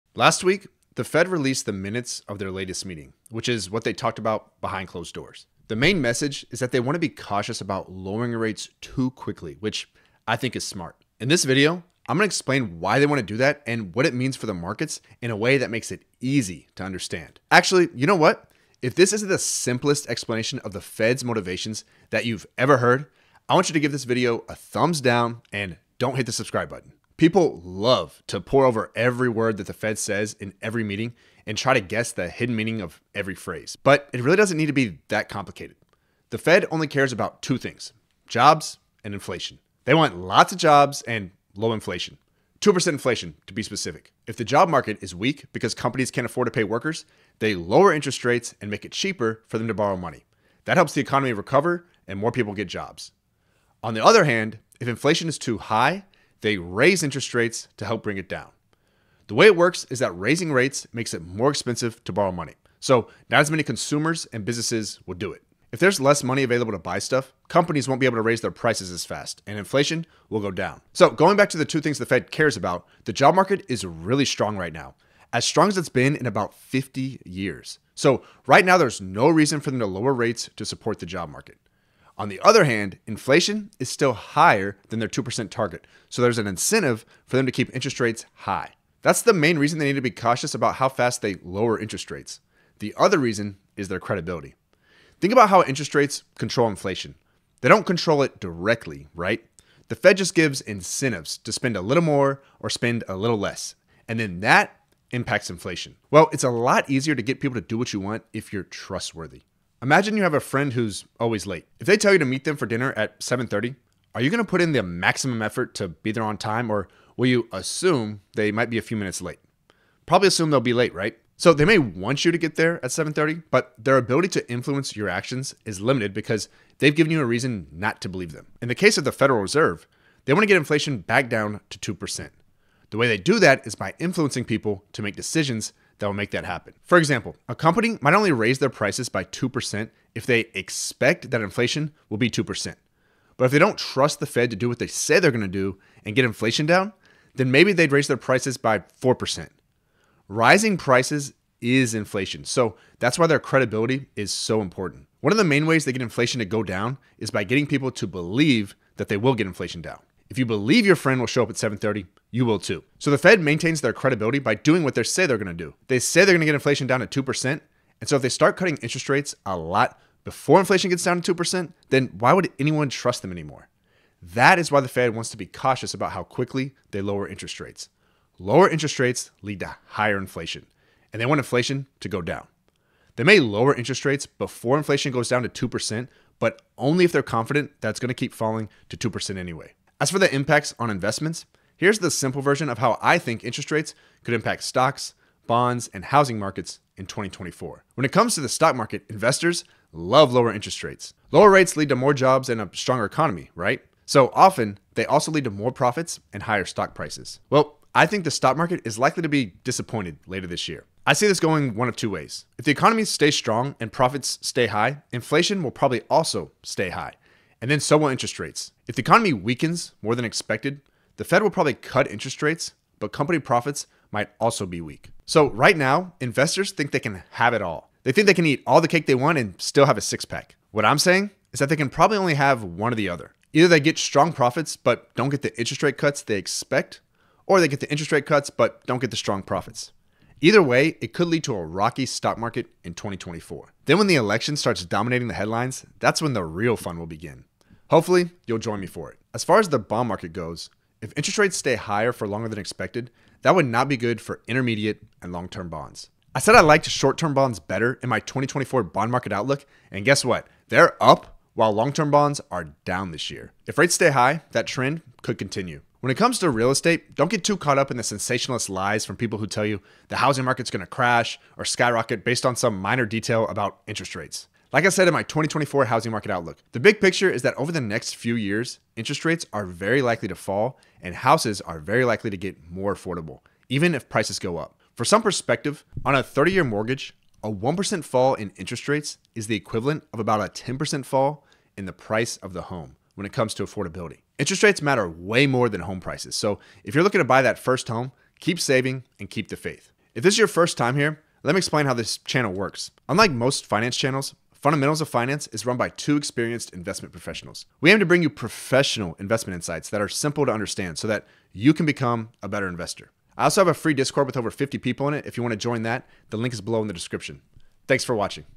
Interest Rates Higher For Longer in 2024? What to Expect
Last week the Fed released the minutes of their latest meeting, which is what they talked about behind closed doors.
The main message is that they want to be cautious about lowering rates too quickly, which I think is smart. In this video I’m going to explain why they want to do that and what it means for the markets in a way that makes it EASY to understand. People LOVE to pour over every word that the Fed says in every meeting and try to guess the hidden meaning of every phrase… but it really doesn’t need to be that complicated.
The Fed only cares about 2 things: jobs and inflation.